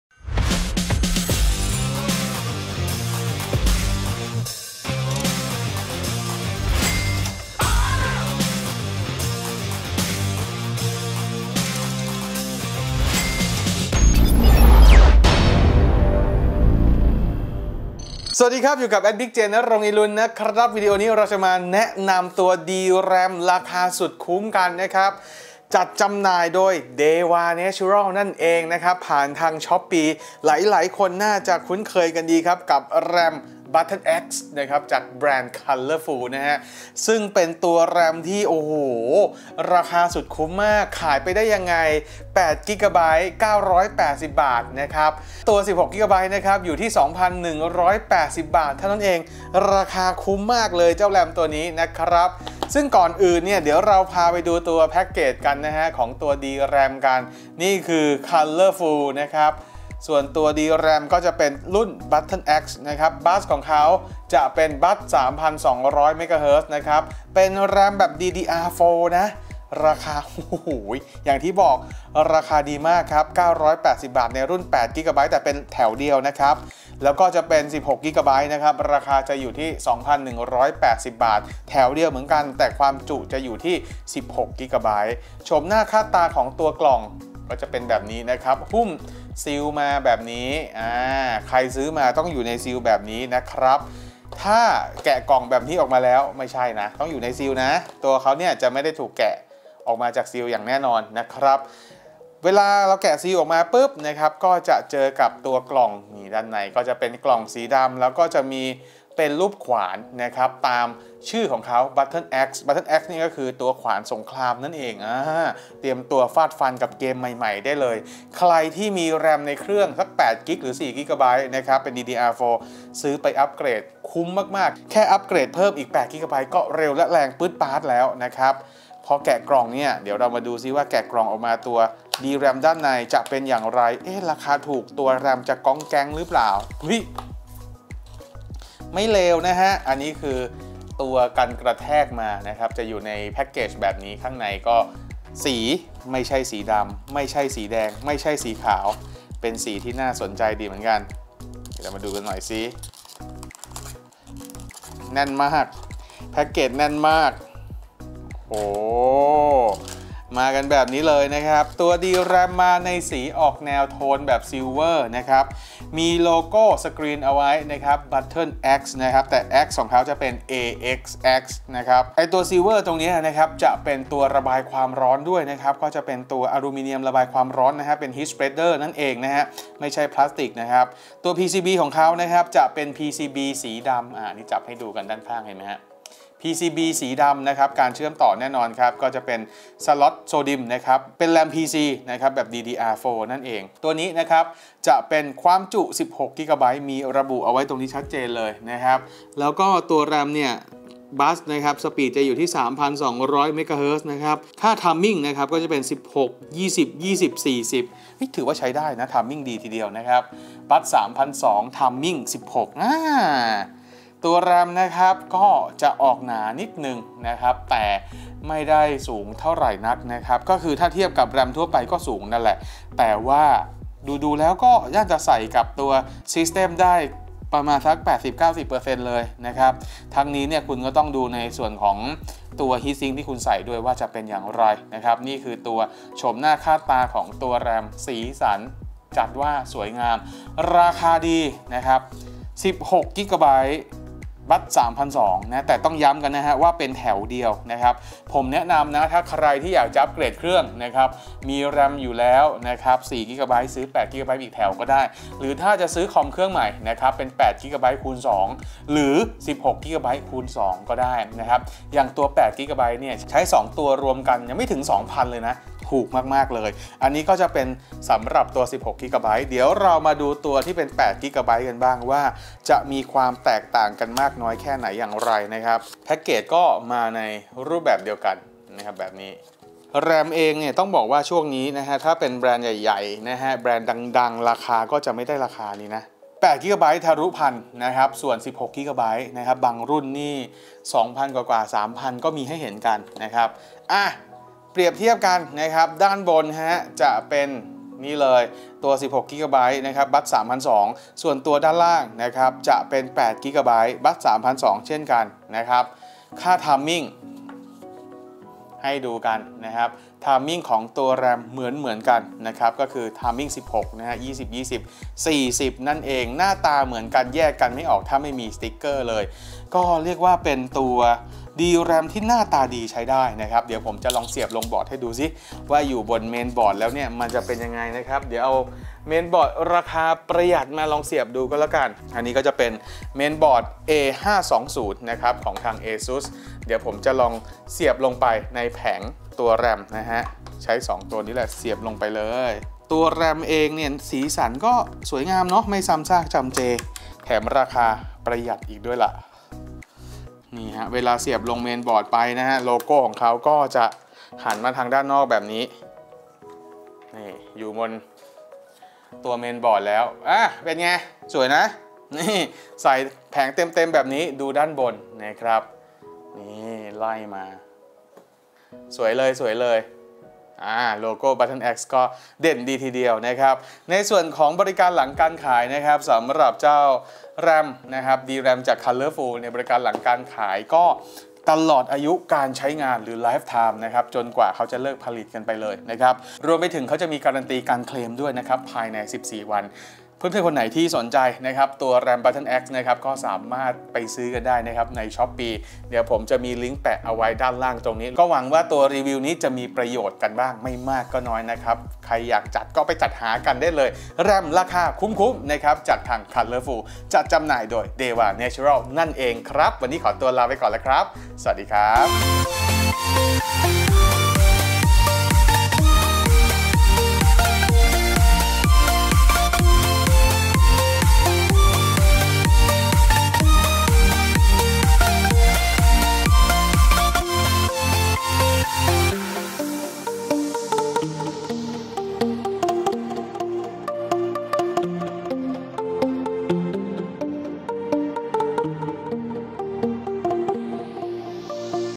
Colorful Battle-AX DDR4 แรมสุดคุ้ม ราคาถูกพิเศษ 8GB-16GB 3200 เริ่มที่ 980 บาท
980 บาท - Colorful RAM สำหรับ PC รุ่น Battle-AX แบบ DDR4 บัส 3200 - CL16 ขนาด 1x8 GB รับประกัน LT - Deva's Ram
2,180 บาท - Colorful RAM สำหรับ PC รุ่น Battle-AX แบบ DDR4 บัส 3200 - CL16 ขนาด 1x16 GB รับประกัน LT - Deva's Ram

ติดตามผลงานของเราได้ที่

ติดต่อรีวิวสินค้า กิจกรรมต่าง ๆ